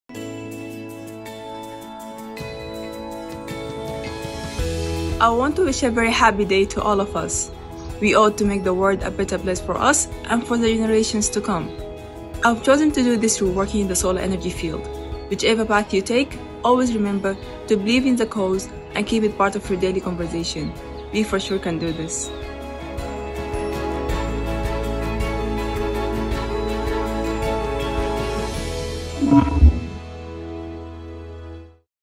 Amna Haroon, an electrical Engineer from Sudan and our Half the Sun scholarship awardee
Marking the opening of International Women's History month, Nextracker applauds and celebrates its women and women across the solar industry and beyond for their social, economic, cultural, and political accomplishments. We'd like to introduce Amna Haroon, an electrical Engineer from Sudan and our Half the Sun scholarship awardee who believes every woman who joins solar makes way for many more."